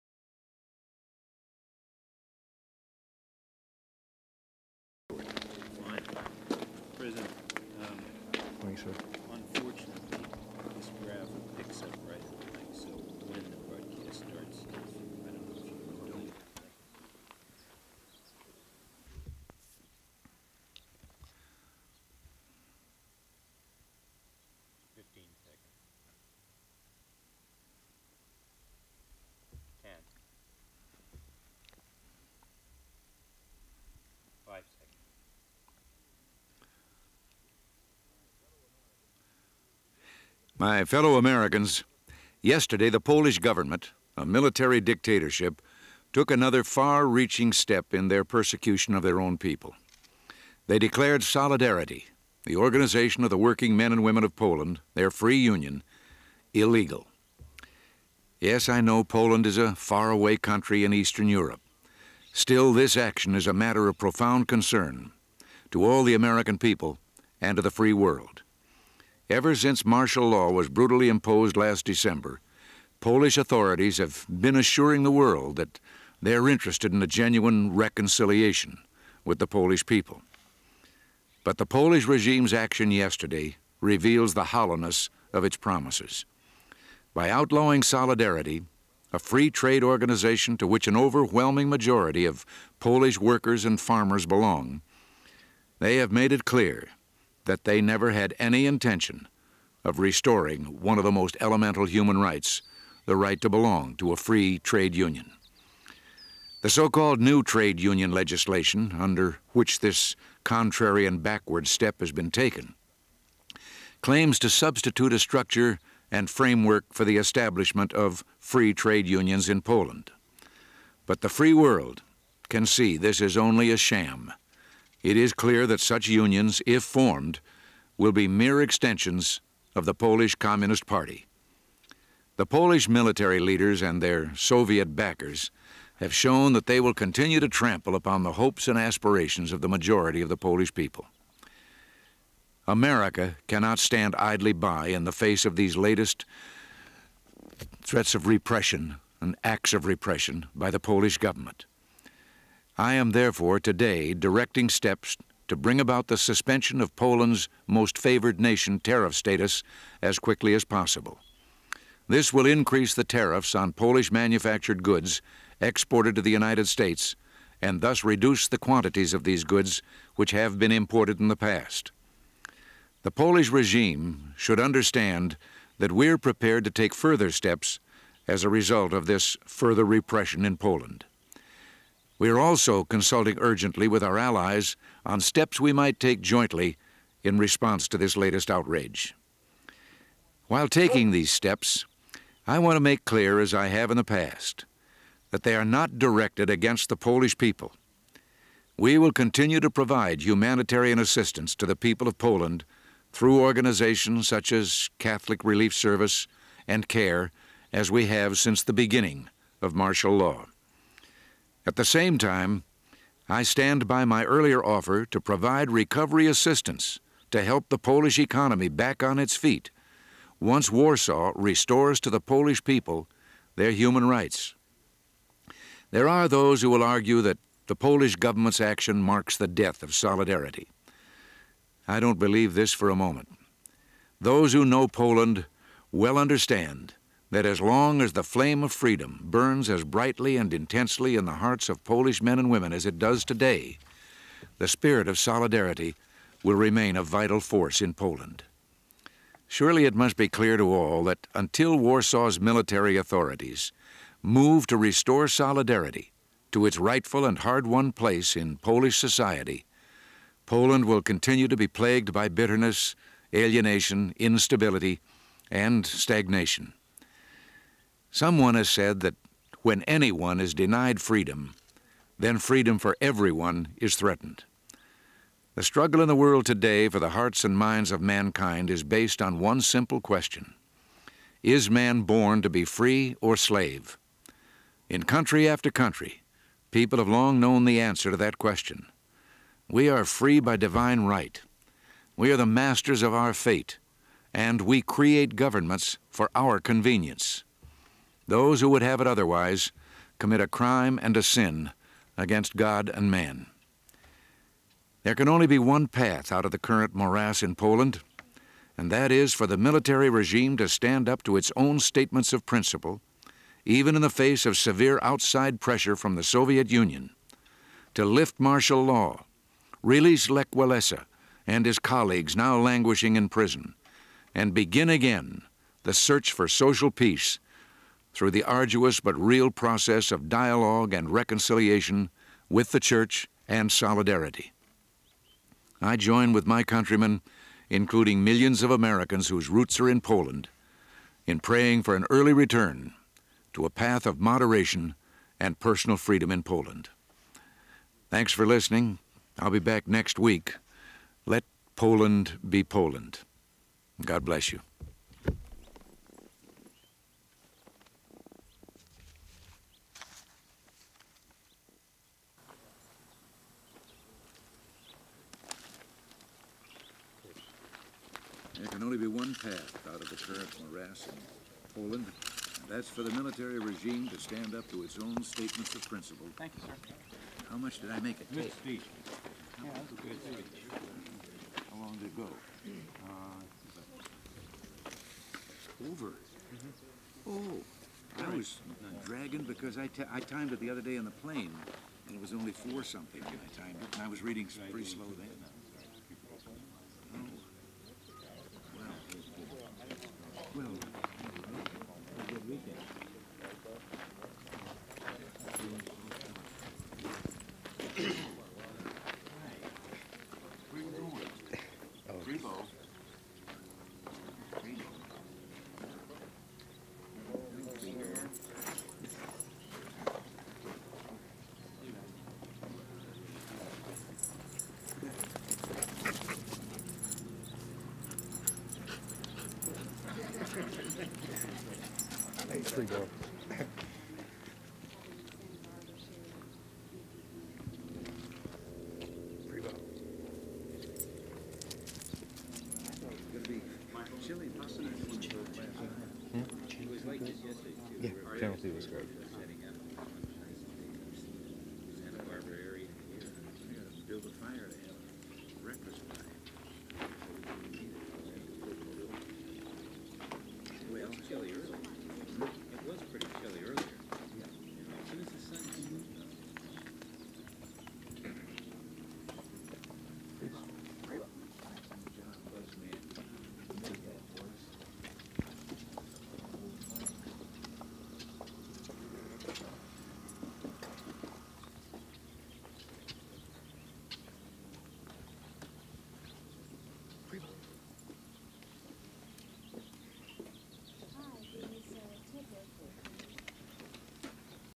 President Reagan Radio Address to the Nation on Solidarity and Poland on October 9, 1982
Full Title: President Reagan Radio Address to the Nation on Solidarity and Poland followed by President Reagan and Freebo the dog outside at Rancho Del Cielo in Santa Barbara, California

Access Restriction(s):Unrestricted
Use Restriction(s):Unrestricted